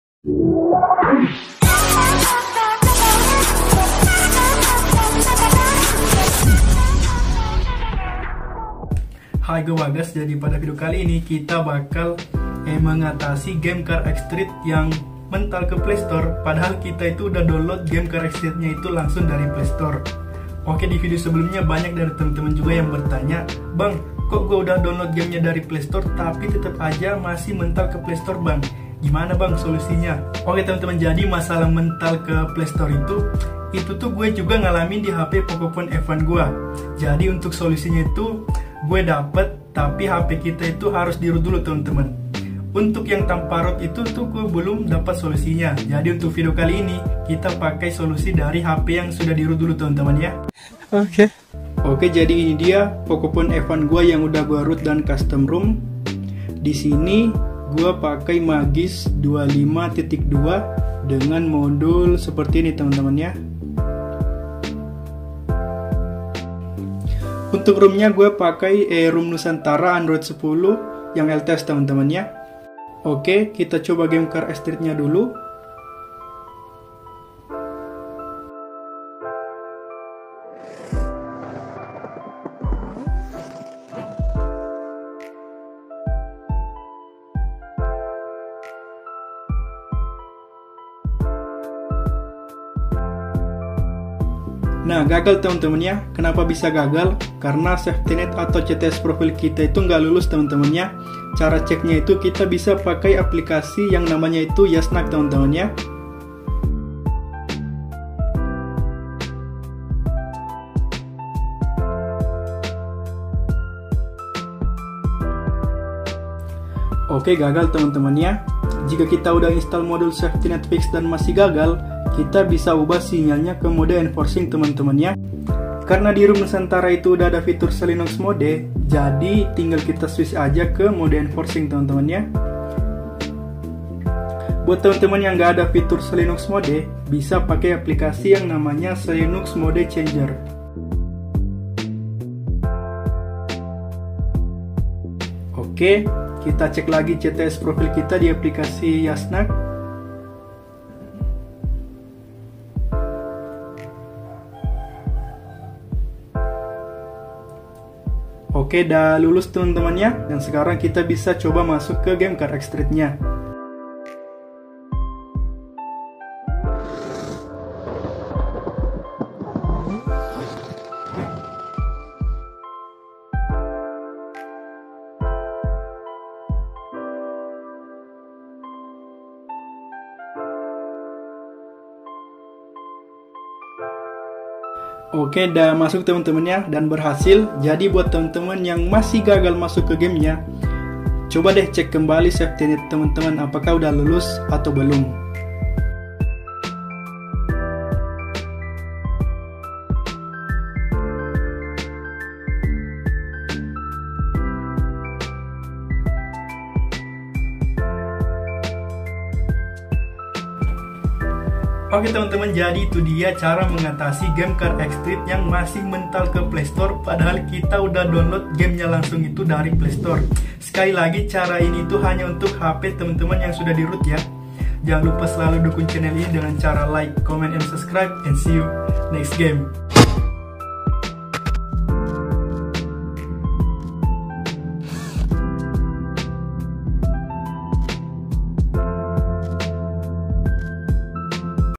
Cara Mengatasi CarX Street 0.8.6 Mental Ke Playstore Padahal Sudah Download Dari PlayStore(Poco F1)
Hi gua Bagas jadi pada video kali ini kita bakal mengtasi masalah game CarX Street yang mental ke Playstore padahal kita udah download game CarX Street nya itu langsung dari Playstore nya.
jadi permasalahan ini itu banyak di tanyakan pada video gua yang sebelumnya dan gua jawab di video ini  ya teman-teman jadi perhatikan baik-baik.
seperti biasa jika ada yang salah atau kurang jelas di video gua teman-teman bisa berkomentar di kolom komentar kita diskusi bareng biar yang lain juga pada tau.

Noted: Untuk modul nya teman2 install aja yang Safetynet, Zygisk dan Magisk Hide sisanya cuman buat ningkatin performa game aja.

Modul yang gua pakai:
1. Universal SafetyNet Fix v2.4.0:
2. Shamiko-v0.6-126-release:
5.UnityFix
6.Super Overlocking Response (120Hz)
7.lyb big. LITTLE force
8.FSTRIM FOR ANDROID
9. FPS-EXPERIENCE

Spesifikasi minimum untuk main CarX Street di Android:
- Android 9.0 and Up
- Prosessor Snapdragon 720G atau setara
- GPU minimal Adreno 618 atau sejenisnya
- Ram minimal 6 GB
- Memori internal 64 GB
- Layar HD 60 Hz